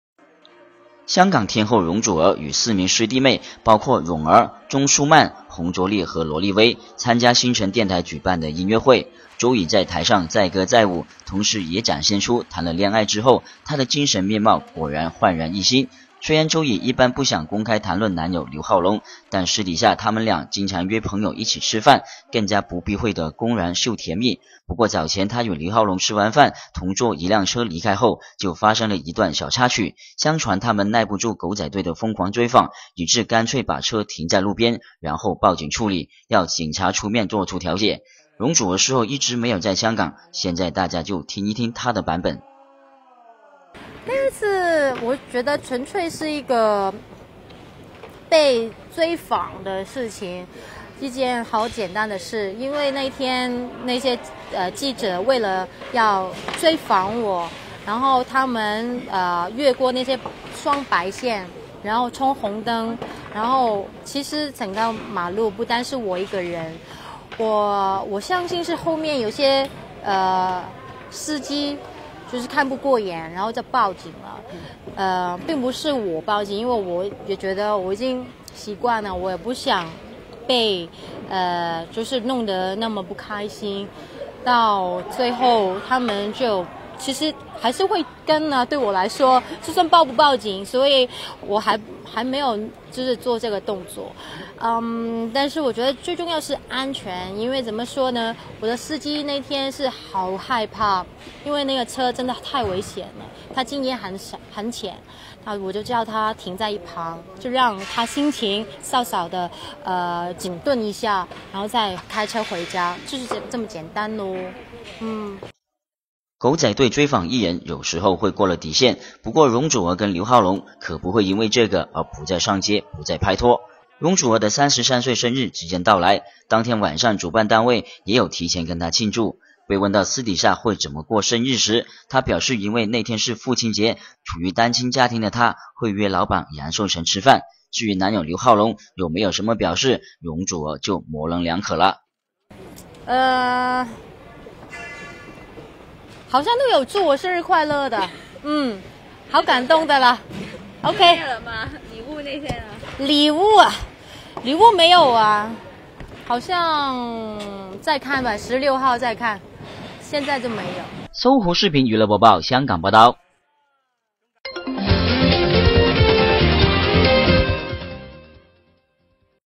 《娱乐资讯》容祖儿否认报警避撇仔队 33生日刘浩龙还没表示
香港天后容祖儿与四名师弟妹包括泳儿 钟舒漫 洪卓立和罗力威参加新城电台举办的音乐会Joey在台上载歌载舞同时也展现出谈了恋爱之后她的精神面貌果然焕然一新虽然Joey一般不想公开谈论男友刘浩龙但是私底下他们俩经常约朋友一起吃饭更加不避讳的公然秀甜蜜不过早前她与刘浩龙吃完饭同坐一辆车离开后就发生了一段小插曲相传他们耐不住狗仔队的疯狂追访以致干脆把车停在路边然后报警处理要警察出面作调解容祖儿事后一直没有在香港现在大家就听一听她的版本